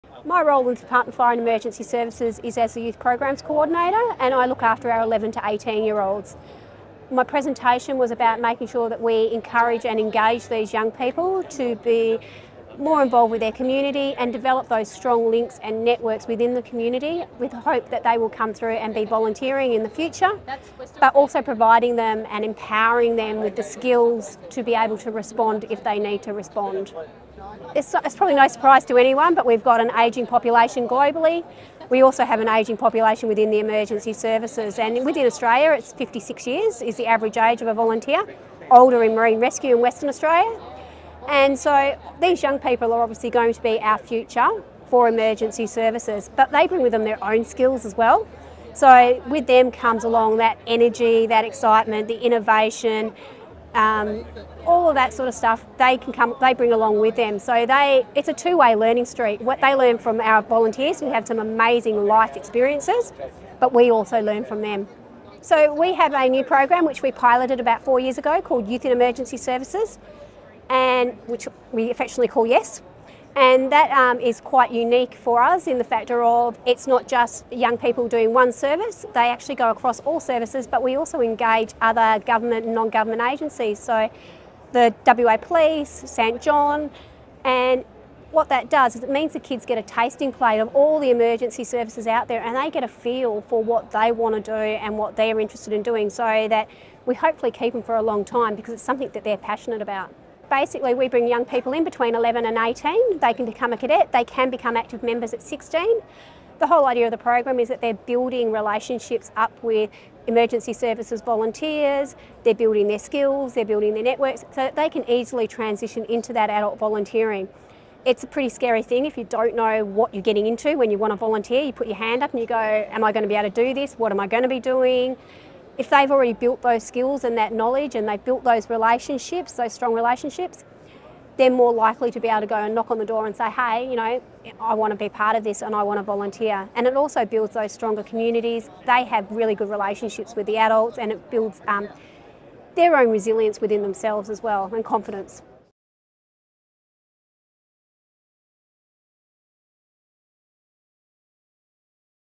The future of volunteering | Lynelle Fozard
Lynelle Fozard, Department of Fire and Emergency Services, WA

Emergency services volunteers come from all walks of life, bringing different skills and insights along with them. Studies show young people are increasingly looking for ways to give back to their community in ways that are meaningful in their personal lives.

The Department of Fire and Emergency Services (DFES) recognises the contribution young people can provide their community. Through the DFES Youth in Emergency Services (YES) program, young people aged 11 to 18 years develop introductory level emergency services skills, are empowered to prevent, prepare and respond appropriately to the hazards that impact them in their community.

In this presentation, Lynelle discusses the history of the program's development, its success to date, and plans for the future of the YES program.